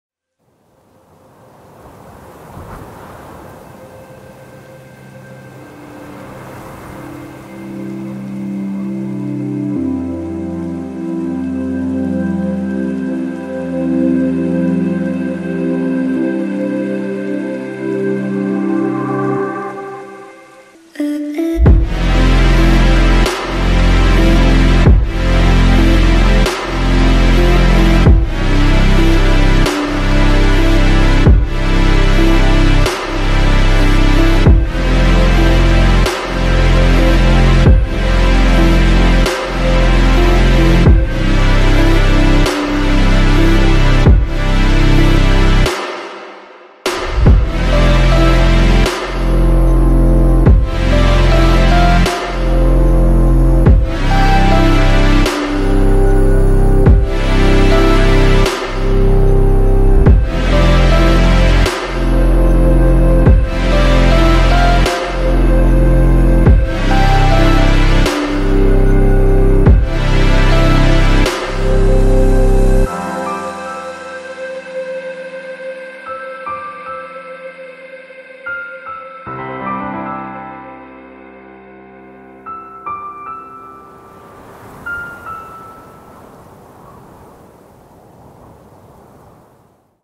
FLY // EP1 // China & Hong Kong // (droning the tallest building in china)
An aerial cinematic short film, shot in Hong Kong, Shenzhen, Guangzhou & Shanghai.